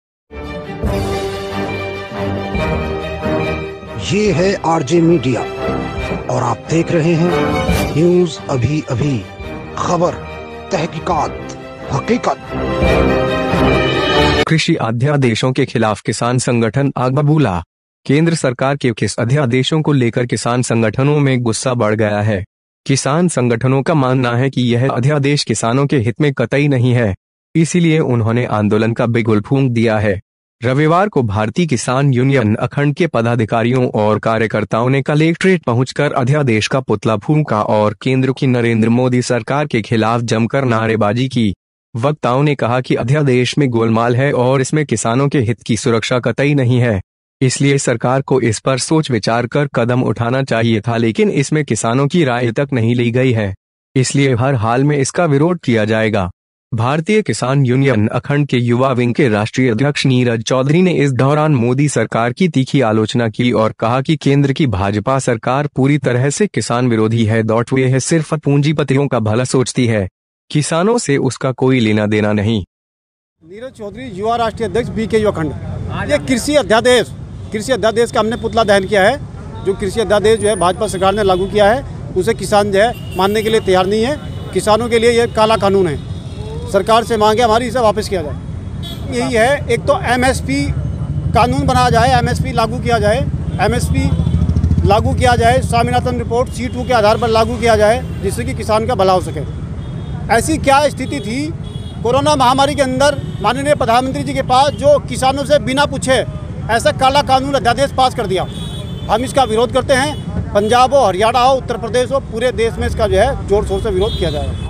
कृषि अध्यादेश पर किसानों का फूटा गुस्सा, देखिए किसका फूंका पुतला
कृषि आध्यादेशों के खिलाफ किसान संगठन आगबबूला. केंद्र सरकार के किस अध्यादेशों को लेकर किसान संगठनों में गुस्सा बढ़ गया है. किसान संगठनों का मानना है कि यह अध्यादेश किसानों के हित में कतई नहीं है. इसीलिए उन्होंने आंदोलन का बिगुल फूंक दिया है. रविवार को भारती किसान यूनियन अखंड के पदाधिकारियों और कार्यकर्ताओं ने कलेक्ट्रेट पहुंचकर अध्यादेश का पुतला फूंका और केंद्र की नरेंद्र मोदी सरकार के खिलाफ जमकर नारेबाजी की. वक्ताओं ने कहा कि अध्यादेश में गोलमाल है और इसमें किसानों के हित की सुरक्षा कतई नहीं है. इसलिए सरकार को इस पर सोच विचार कर कदम उठाना चाहिए था लेकिन इसमें किसानों की राय तक नहीं ली गई है. इसलिए हर हाल में इसका विरोध किया जाएगा. भारतीय किसान यूनियन अखंड के युवा विंग के राष्ट्रीय अध्यक्ष नीरज चौधरी ने इस दौरान मोदी सरकार की तीखी आलोचना की और कहा कि केंद्र की भाजपा सरकार पूरी तरह से किसान विरोधी है .वह सिर्फ पूंजीपतियों का
भला सोचती है .
किसानों से उसका कोई लेना-देना नहीं.